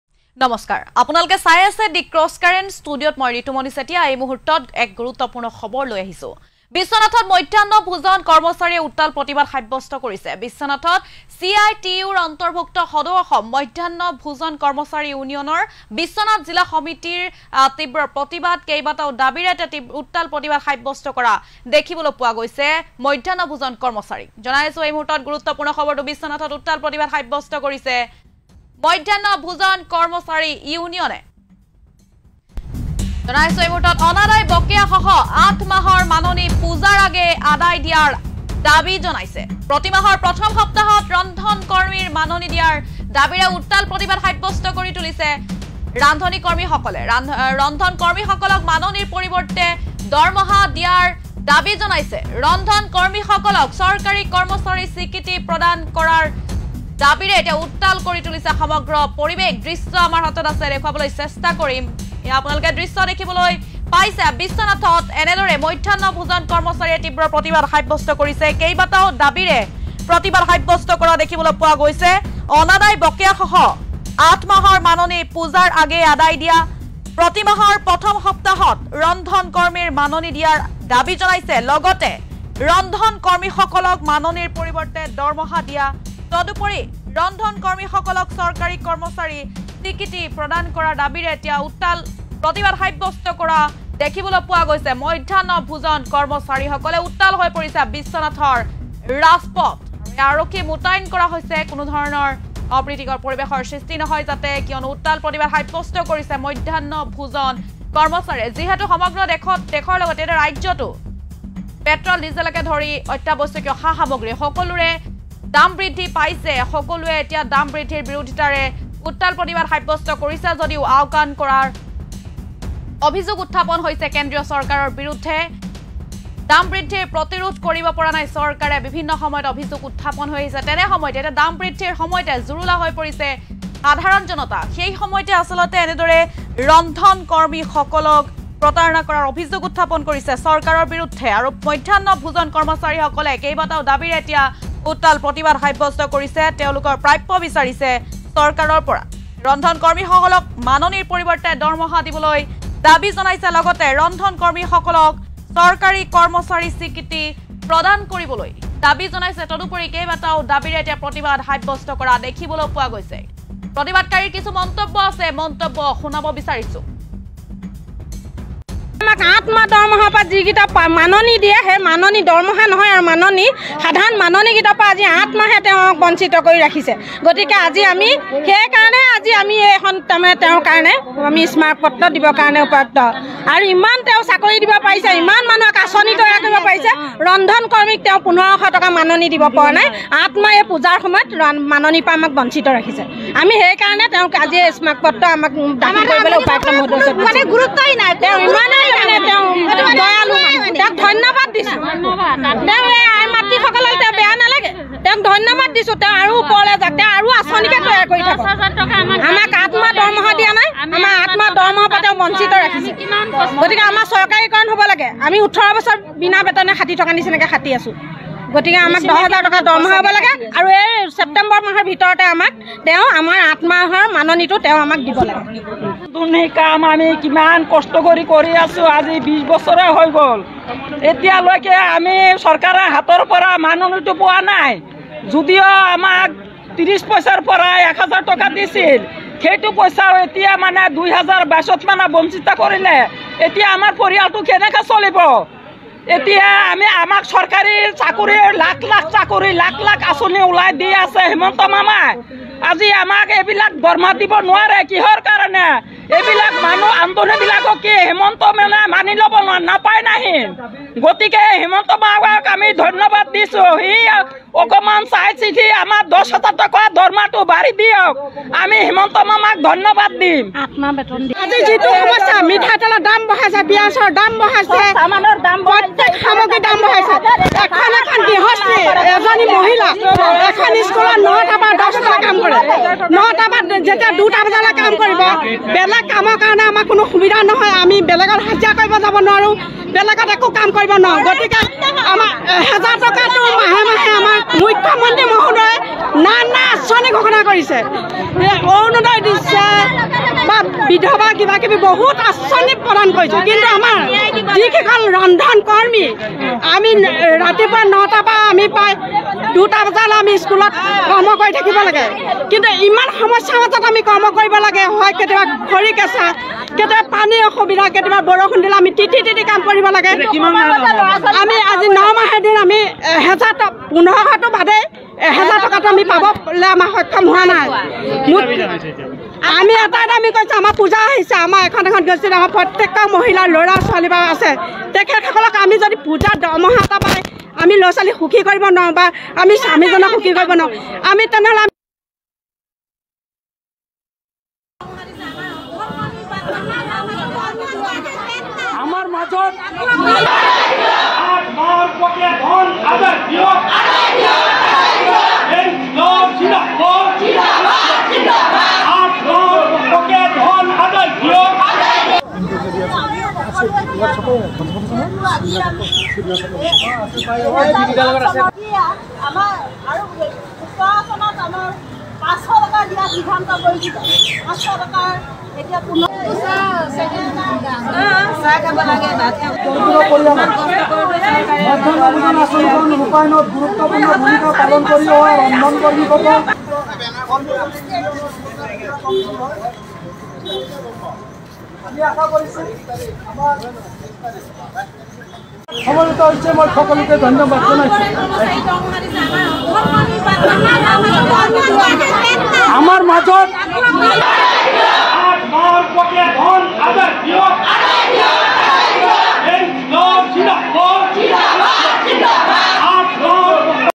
বিশ্বনাথত সদৌ অসম মধ্যাহ্ন ভোজন কৰ্মচাৰী ইউনিয়নৰ প্ৰতিবাদ
অনাদায় বকেয়া সহ ৮ মাহৰ মাননি পূজাৰ আগেয়ে আদায় দিয়া ।
প্ৰতি মাহৰ প্ৰথম সপ্তাহত ৰন্ধন কৰ্মীৰ মাননি দিয়া ।